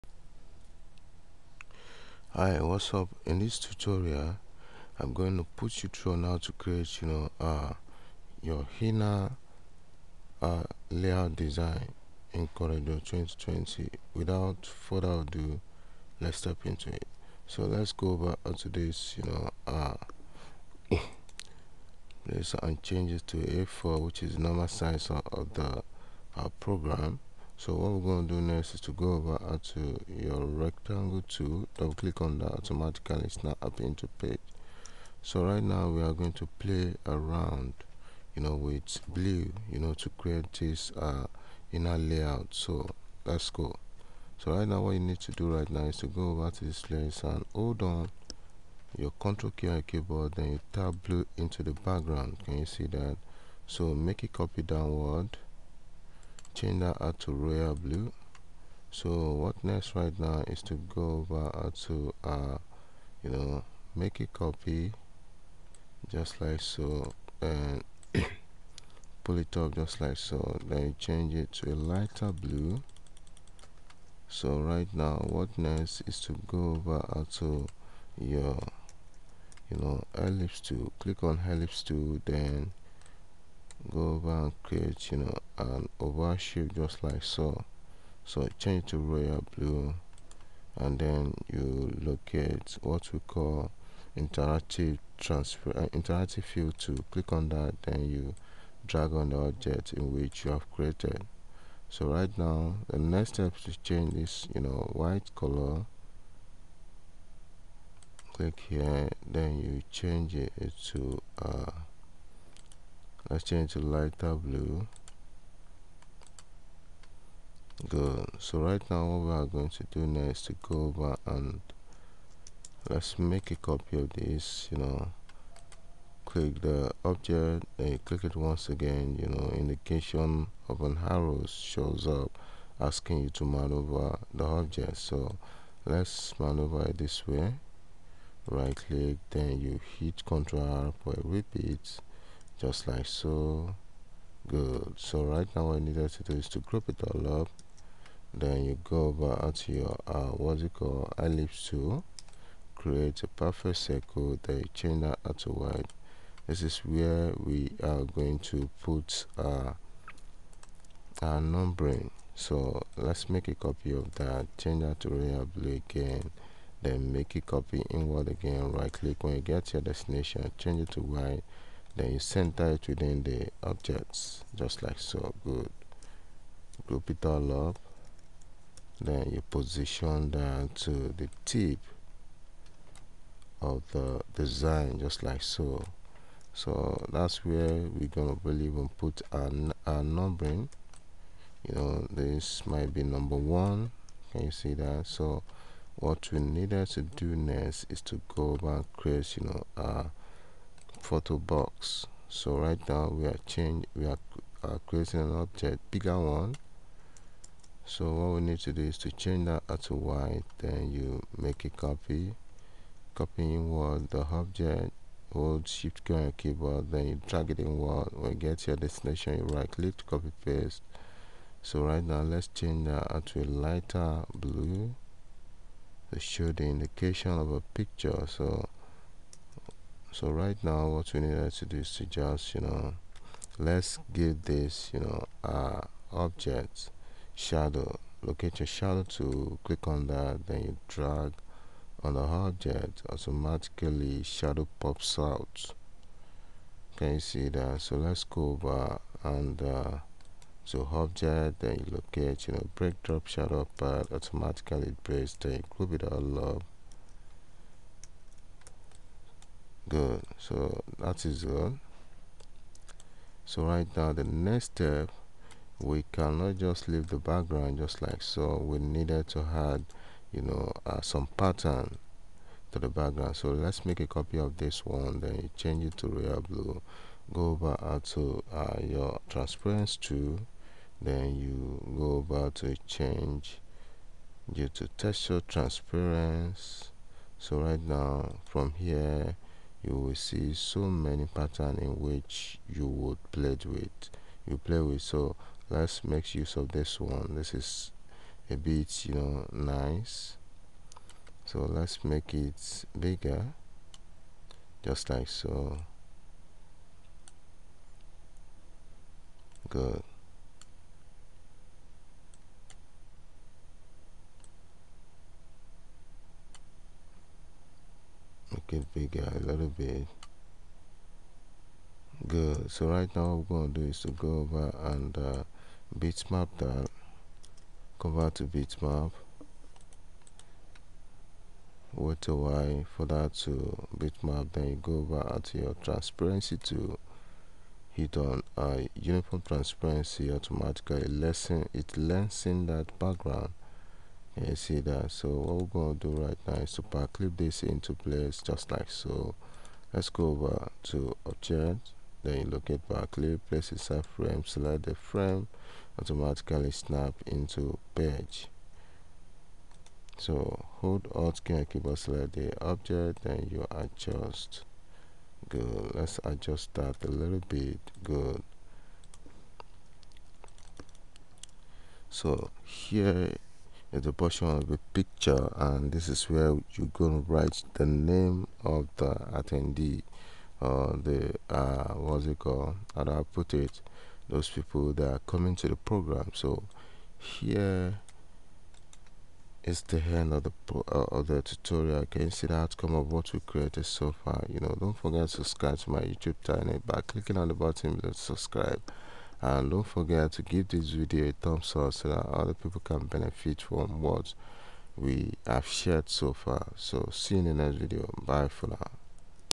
Order of Program Inner Template Design - CorelDraw Tutorial
How to create Order of Program Inner Template Design with CorelDraw.